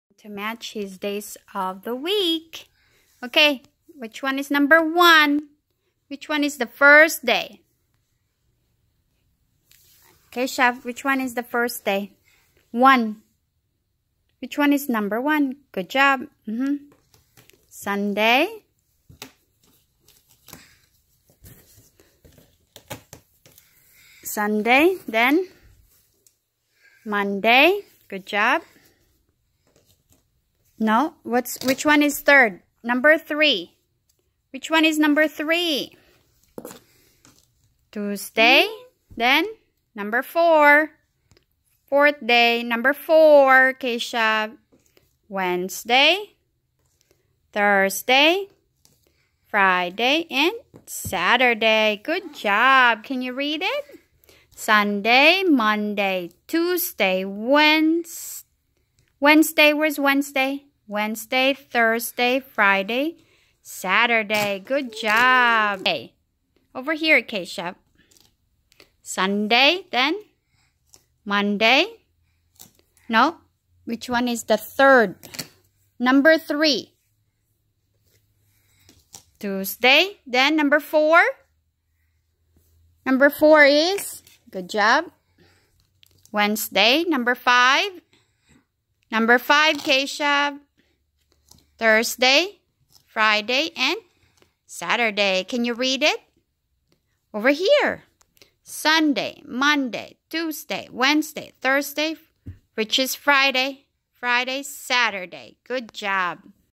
Literacy: Days of the week, Months of the year and Weather(1)
Keshav matches his days, months and weather using identical or non identical matching then Keshav sequences them following a visual prompt of numbers to help Keshav in sequencing it.